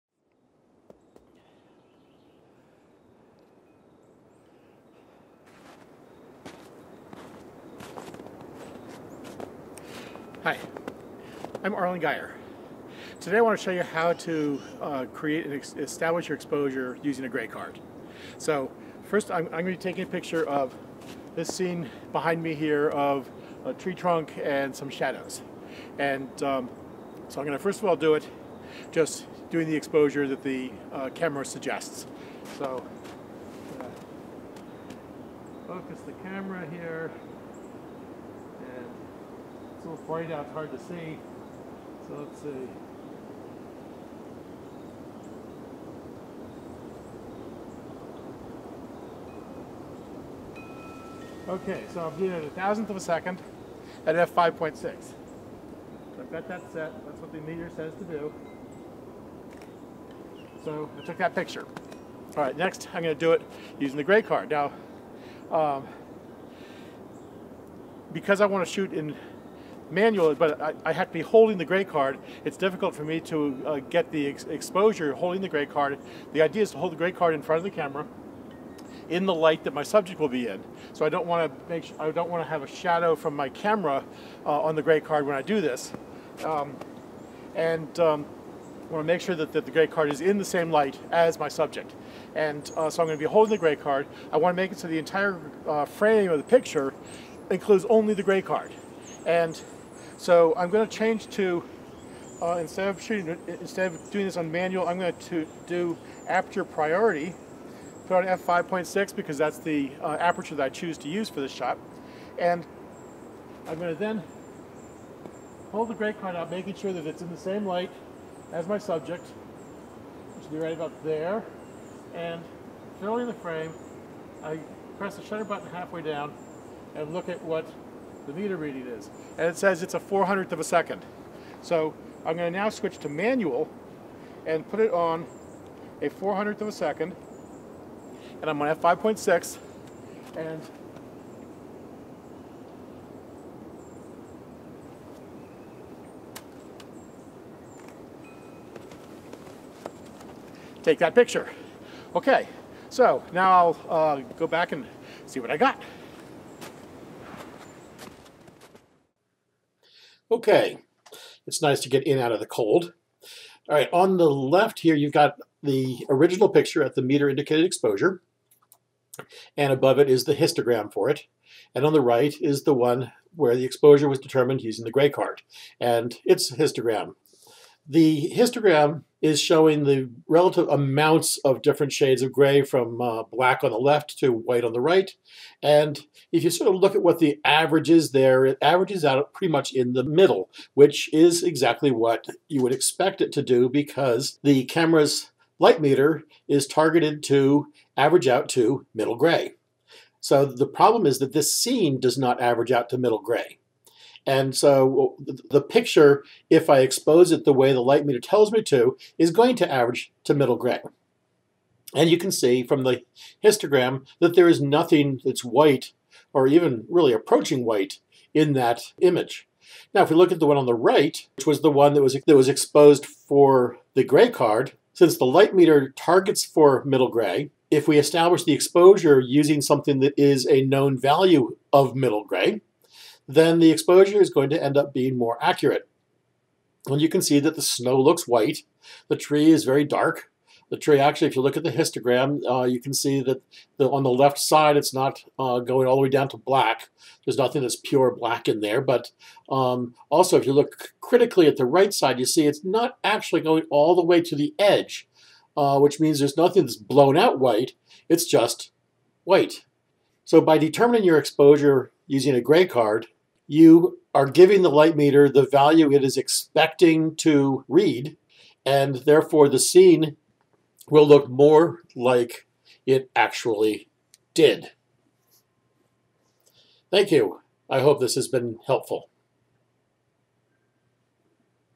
Exposing with a Grey Card
How to determine your camera exposure using an 18% grey card.  Grey cards are available at many local camera stores and online photo merchants.